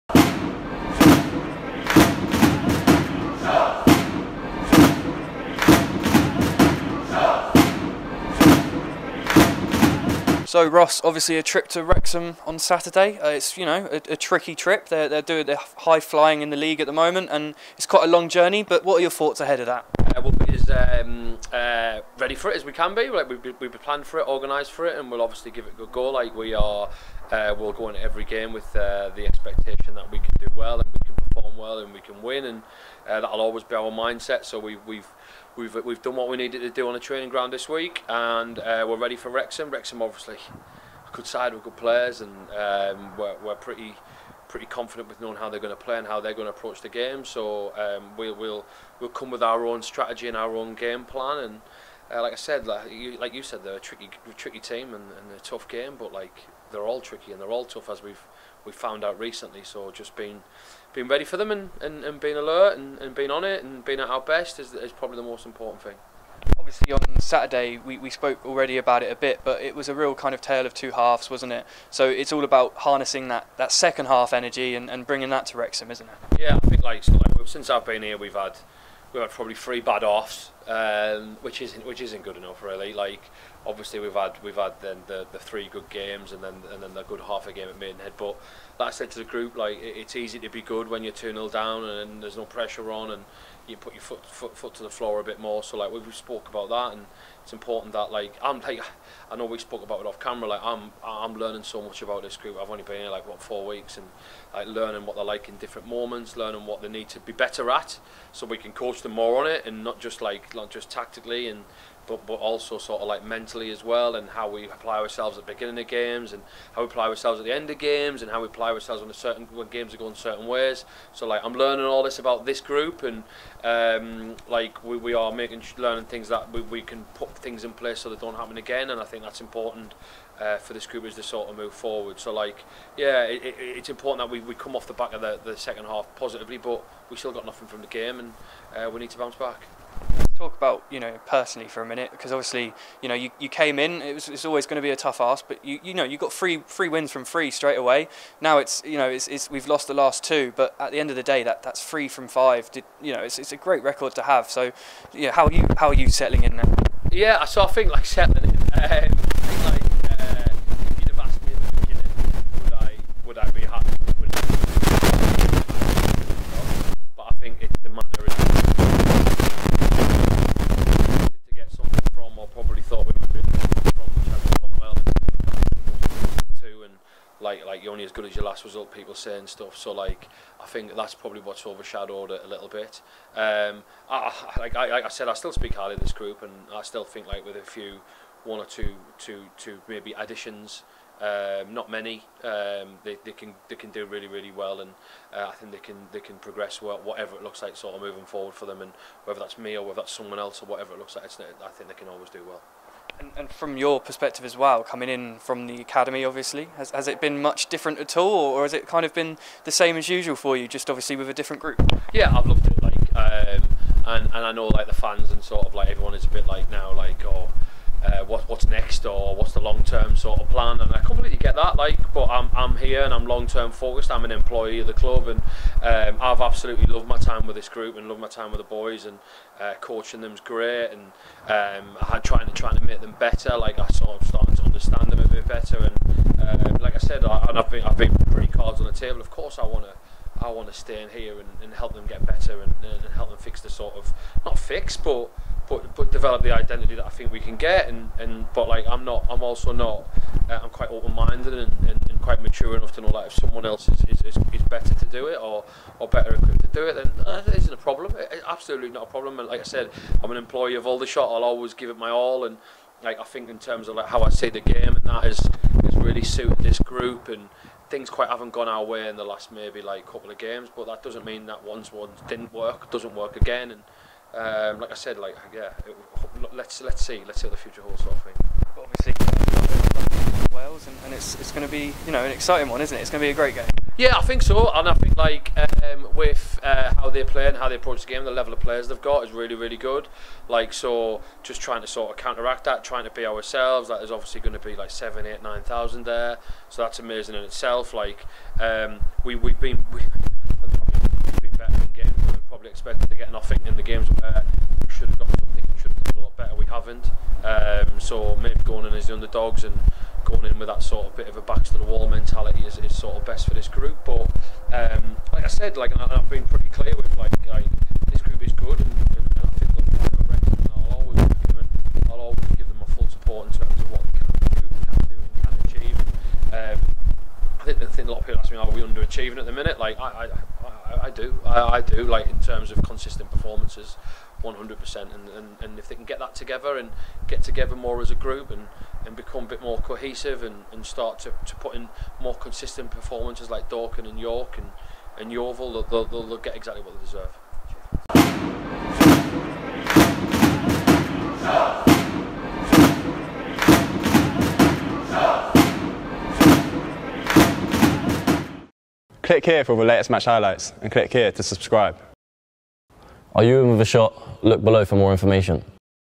ROSS MCNEILLY PRE-MATCH: Wrexham vs Shots
Ross McNeilly previews this weekend's trip to Wrexham!